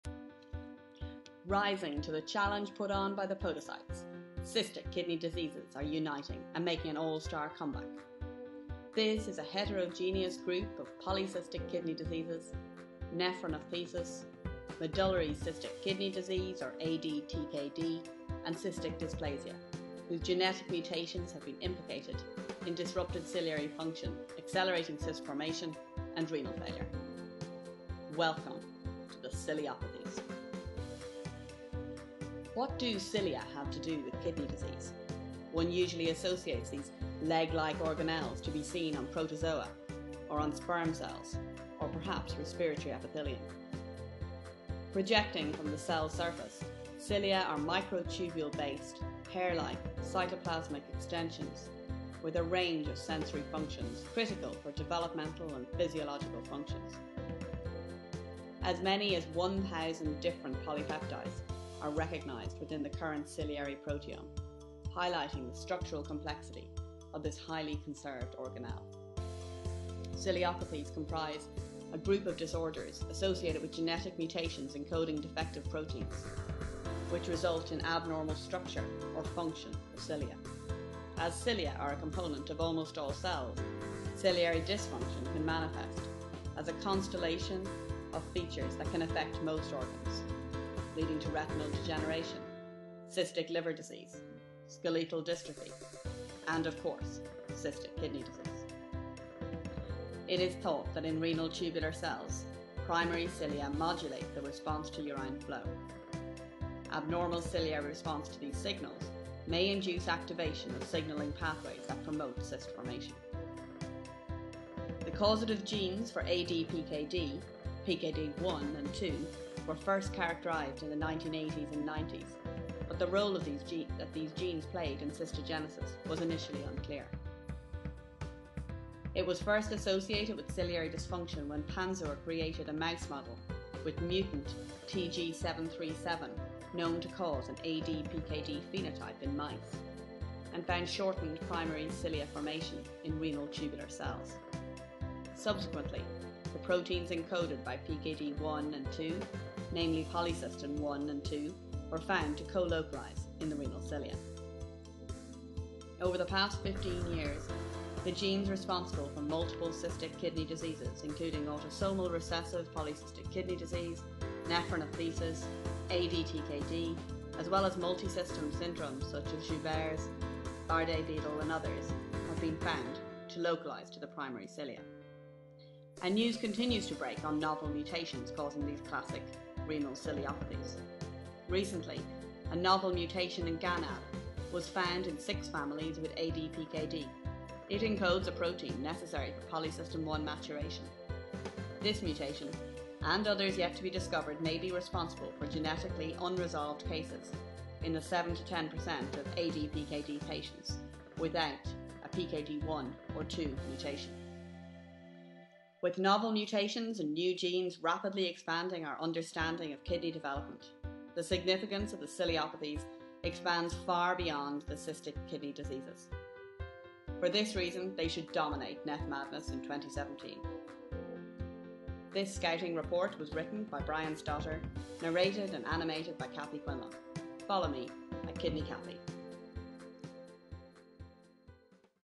NephMadness 2017: Ciliopathy Scouting Report
Podocytopathies vs. Ciliopathies
This rivalry is at the core(tex) of genetic kidney diseases. Mutations affecting podocyte function are pitted against defects in tubular cell cilia function.
Can the podocyte foot processes efface the track record of research underlying cystic kidney diseases? Or will the ciliopathies sweep the podocytes out of the tournament and move on to the next round?